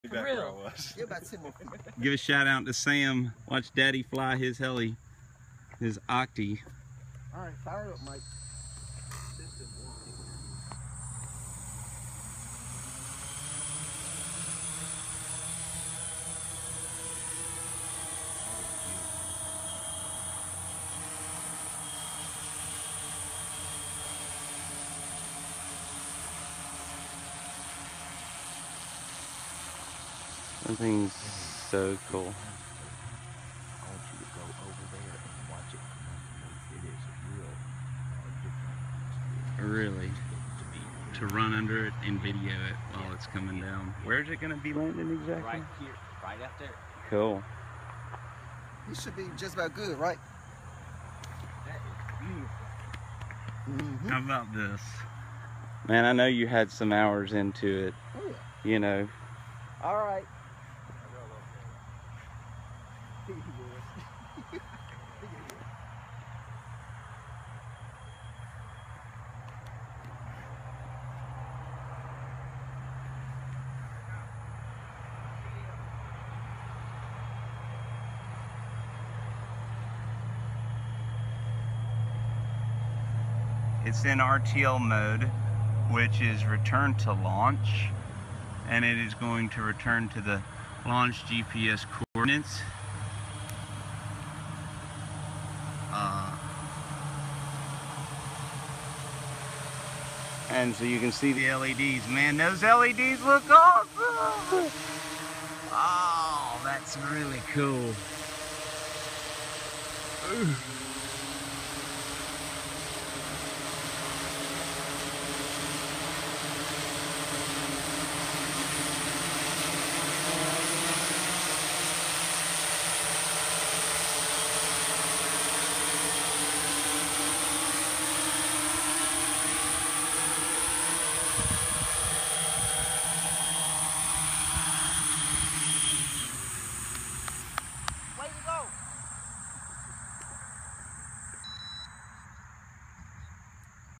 DJI S1000+ Octocopter with 3DR Pixhawk Arducopter
I built this DJI S1000+ with a 3DR Pixhawk flight controller. We run Arducopter firmware with a 6S 20,000mah LiPO battery. This footage was a few flights after the maiden flight, and we were testing out the Return to Launch flight mode. I fly the drone out to about 800ft and an altitude of about 30 meters, flip the switch, and the drone autonomously flies back to the GPS coordinates of liftoff, then begins a slow decent and lands itself. This is great technology, and we haven't even begun to scratch the surface of possibilities.